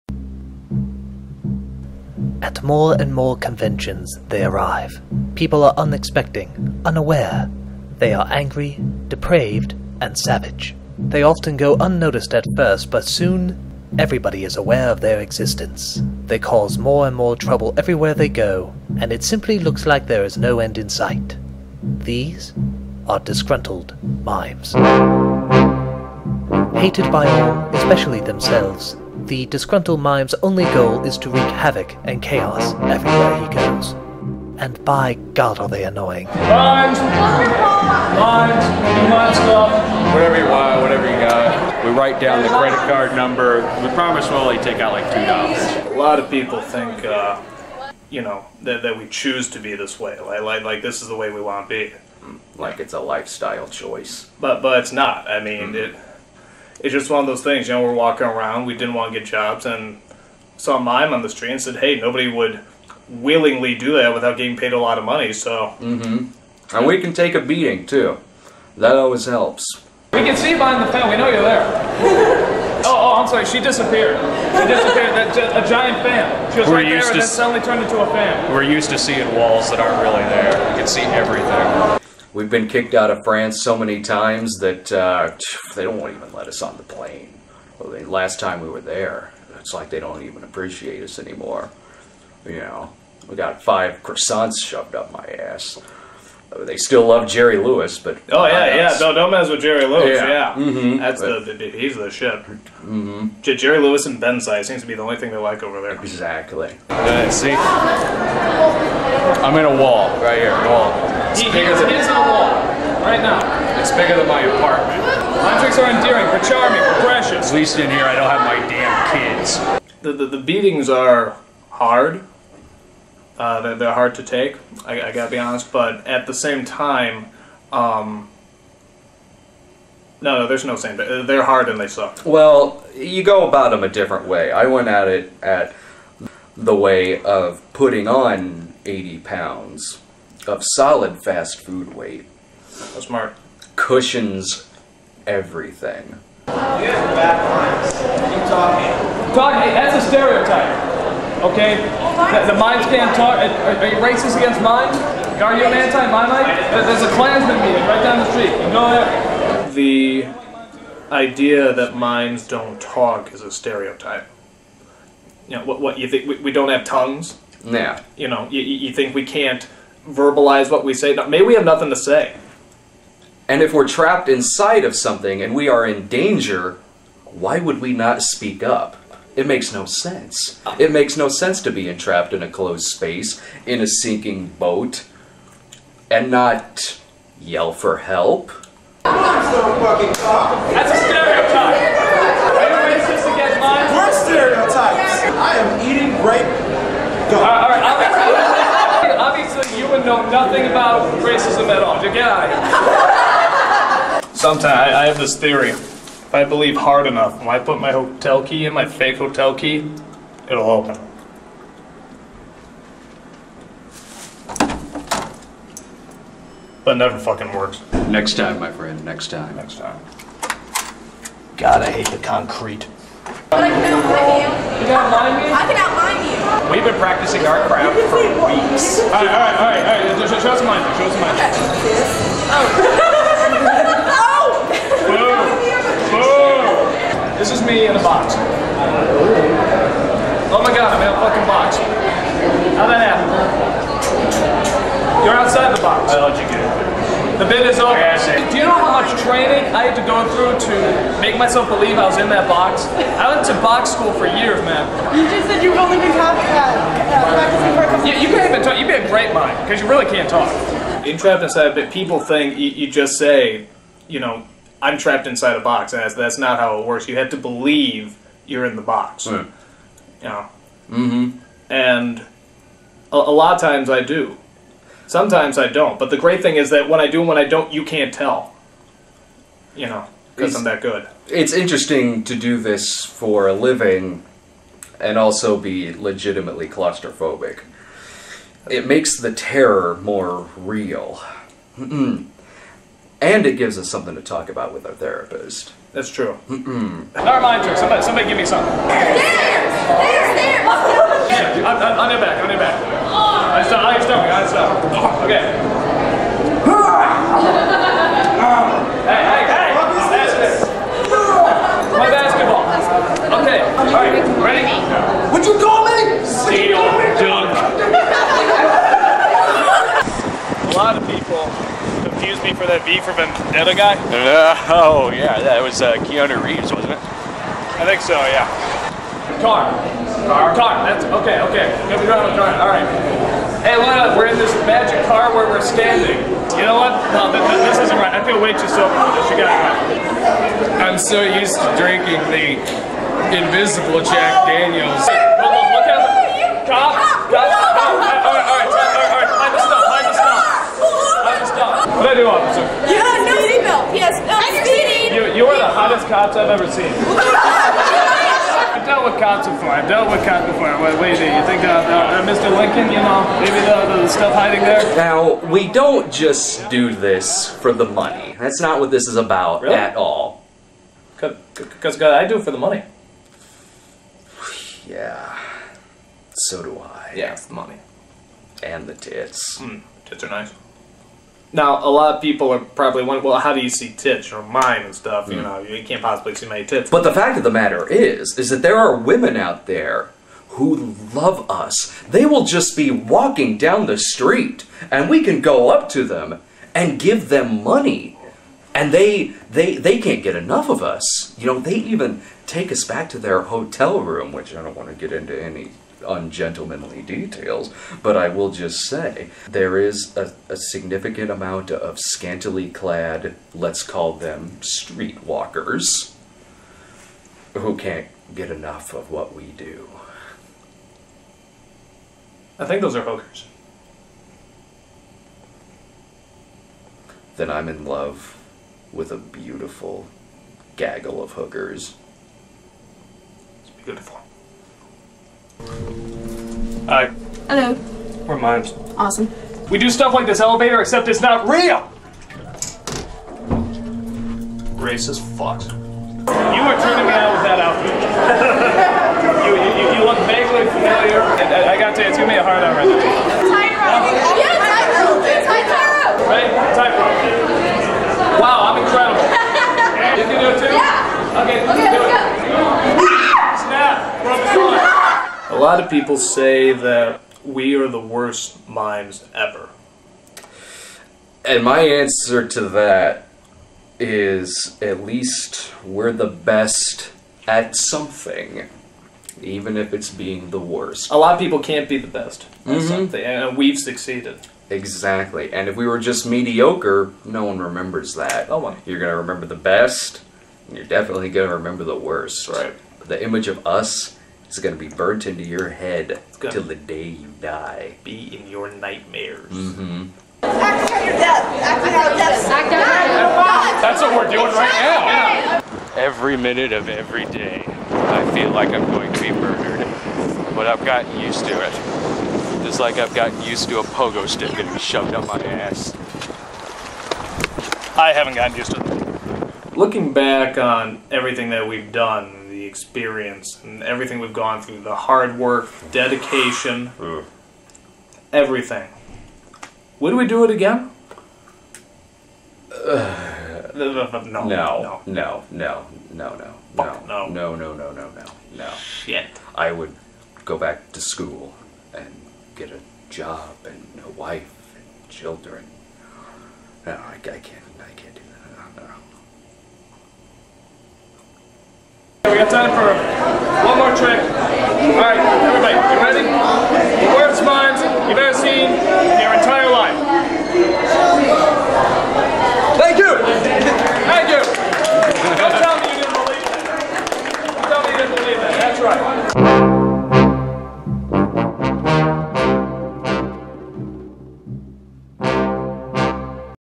Sketch: Disgruntled Mimes with Brad Jones
Doug and Brad are disgruntled mimes.